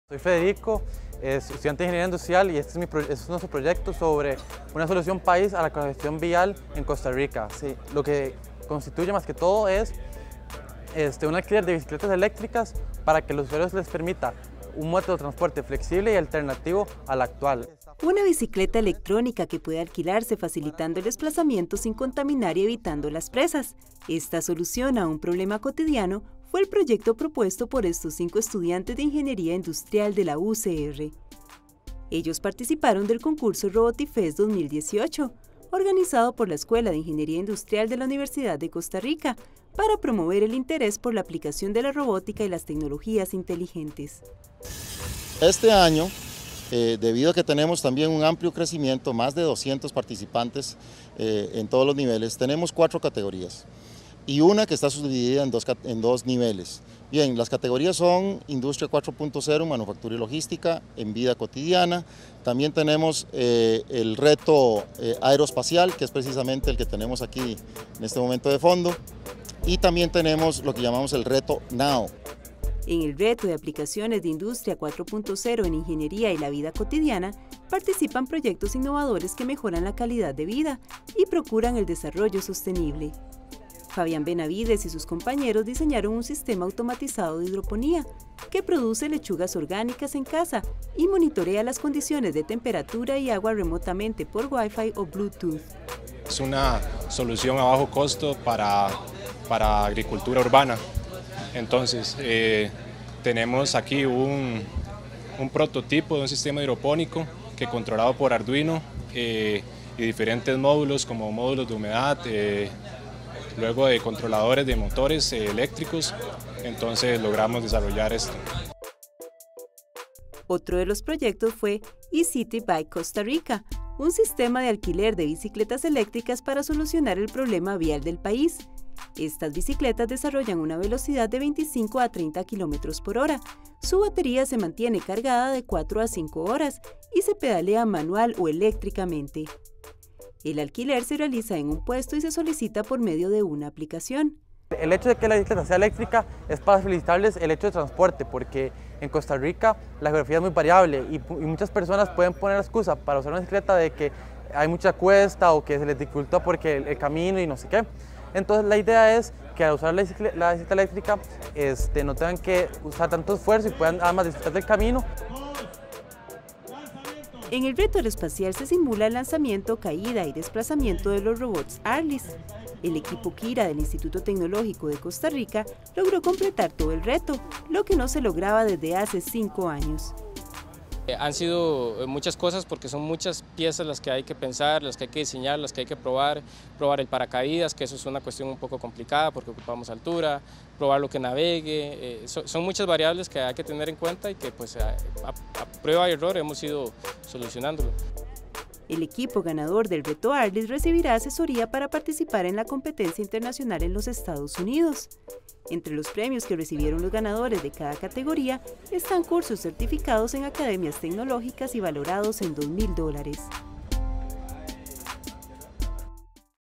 Proyectos innovadores para problemas cotidianos y el reto aeroespacial presentes en Robotifest 2018
Jóvenes universitarios entusiastas de la robótica y las tecnologías inteligentes se dieron cita en el concurso RobotiFest UCR 2018, organizado por la UCR. Soluciones para cultivar lechugas en casa y una bicicleta electrónica fueron algunas de las propuestas de los estudiantes de Ingeniería Mecánica.
El reto del lanzamiento de los robots ARLISS desde 40 metros de altura fue una actividad que llamó la atención de los asistentes a este evento.